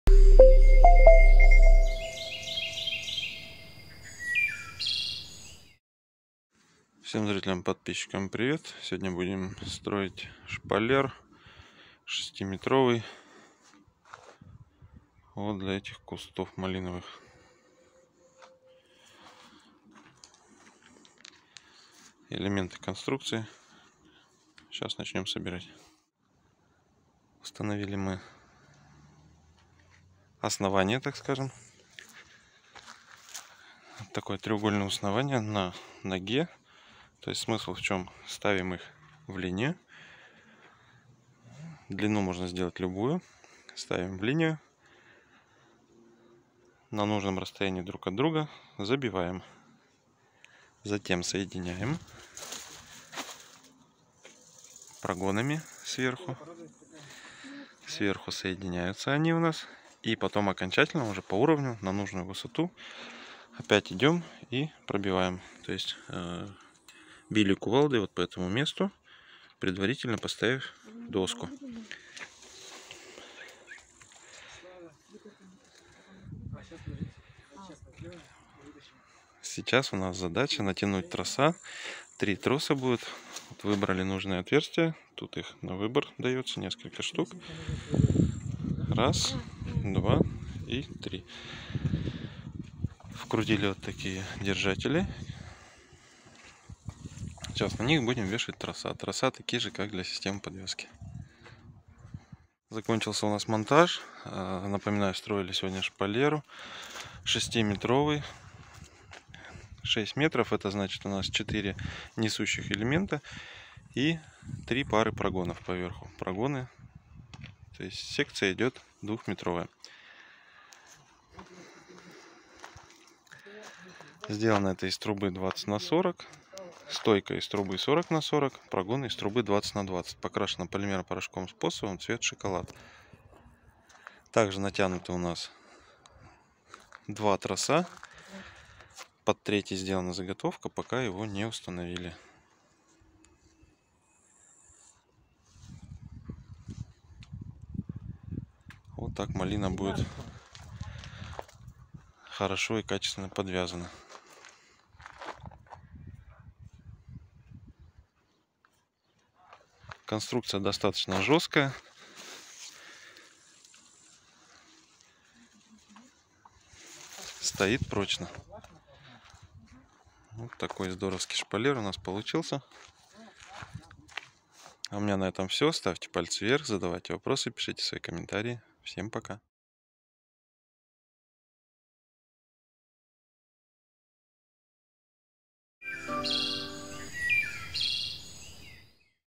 ШПАЛЕРА ДЛЯ МАЛИНЫ ИЛИ СМОРОДИНЫ.
У вас Малина или смородина?

Как подвязать?
Вам нужна конструкция для поддержки и подвязывания.
И это капитальная конструкция называется шпалера.

Шпалера нужна для поддержки растений, равномерного роста и увеличения плодоношения.
В компании Уральская Усадьба,  производят качественные и удобные шпалеры. Они состоят из рогатин, прогонов и фурнитуры. Весь комплект удобно упакован и легко пересылается в любое место.
При получении шпалеры вы легко проведете монтаж самостоятельно. Для этого необходимо рогатины вбить в землю на 40- 50 см и укрепить или залить бетон в подготовленные лунки. На них устанавливаются прогоны и натягивается трос. Вся фурнитура в комплекте, отверстия просверлены заранее.
Мы производим шпалеры из оцинкованной трубы 20/20, 20/40 и 40/40. Для защиты от коррозии применяем порошково-полимерную покраску. Такая конструкция будет стоять очень долго и радовать вас красотой и большим урожаем!

Стоимость комплекта 2 метра: две рогатины, два прогона и два троса с фурнитурой всего 5600 рублей.
Удлинение на 2 метра комплект с двумя тросам-  3400 рублей.
За расчетом и консультацией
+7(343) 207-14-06
🏡 Екатеринбург ул Климовская 23
🖥 ural-usadba.ru